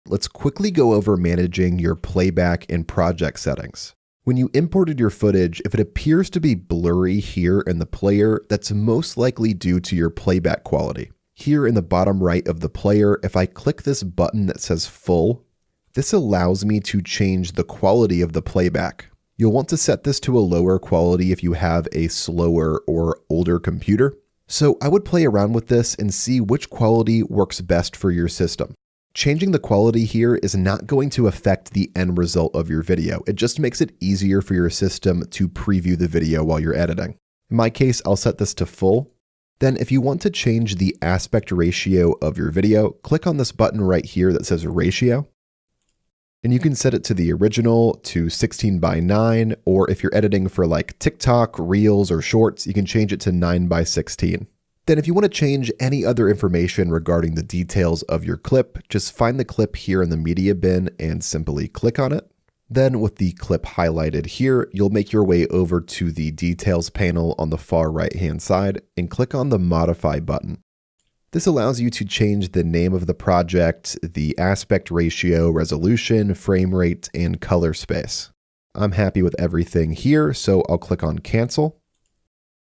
How to Manage Playback & Project Settings in CapCut
Learn how to adjust CapCut project settings for smoother editing! This tutorial covers fixing blurry playback, changing aspect ratio for YouTube or TikTok, and modifying resolution, frame rate, and color space in CapCut.